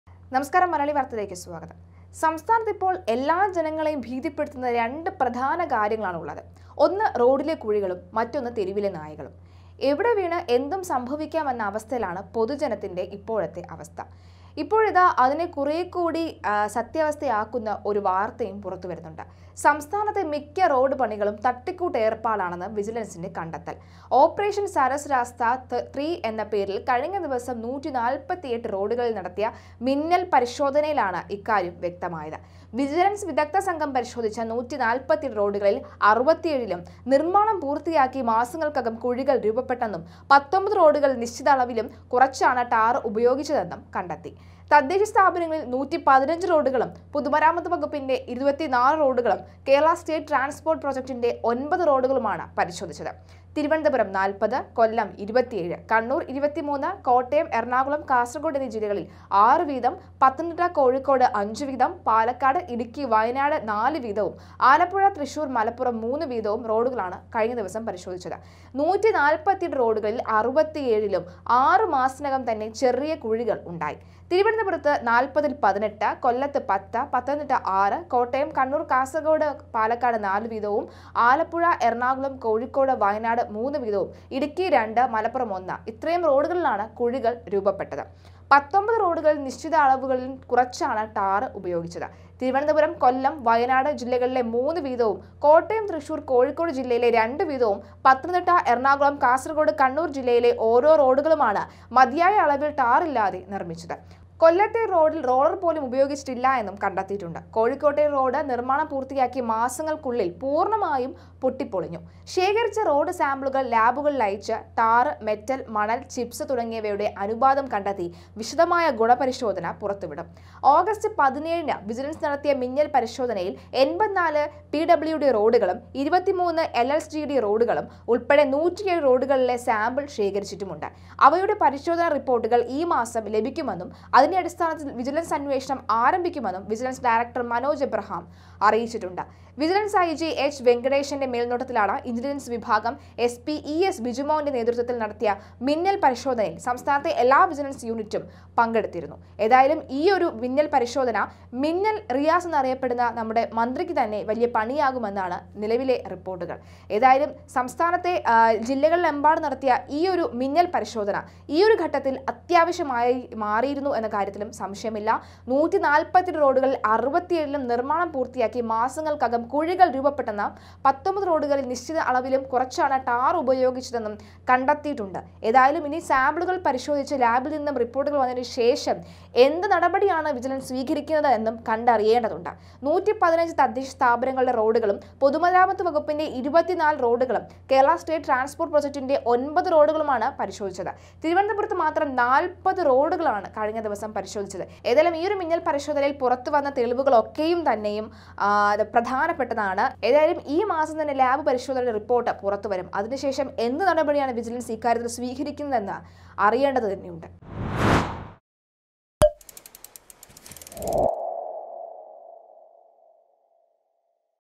Muhammad Riyas റോഡുപണി വെറും തട്ടിക്കൂട്ടെന്ന് വിജിലന്‍സ്
വിജിലൻസ് കളത്തിൽ